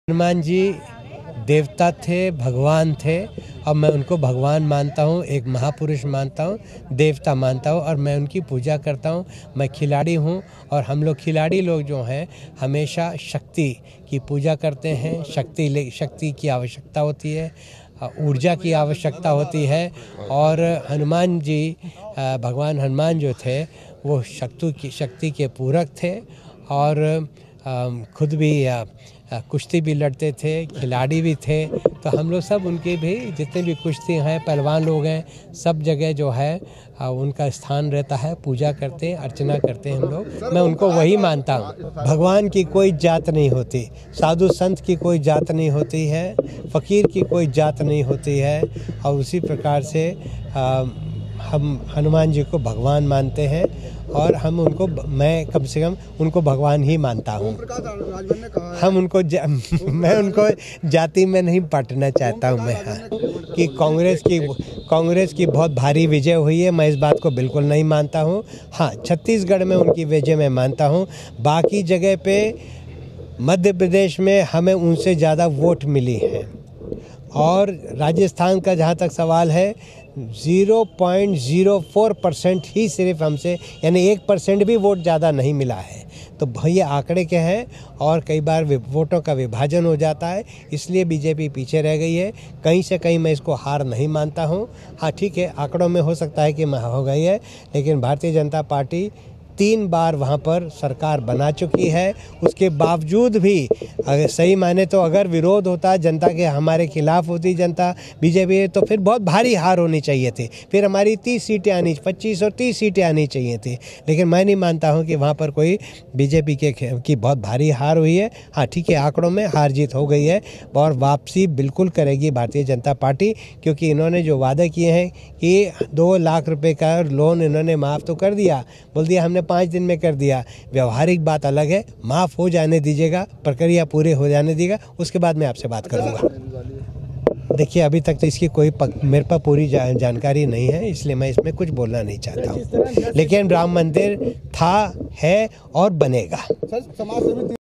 'मैं हनुमान जी की पूजा करता हूं, उनको किसी जाति में नहीं बांध सकता'
अमरोहा: हनुमान जी की जाति को लेकर शुरू हुआ विवाद थमने का नाम नहीं ले रहा है. कैबिनेट मंत्री चेतन चौहान ने भी हनुमान जी पर अपनी बात रखी और कहा कि हनुमान जी भगवान थे, महापुरुष थे और वह उनको भगवान की तरह ही पूजते हैं. वह उनको जाति में नहीं बांध सकते. चेतन चौहान बढ़ती ठंड के चलते गरीबों को कंबल बांटने पहुंचे थे.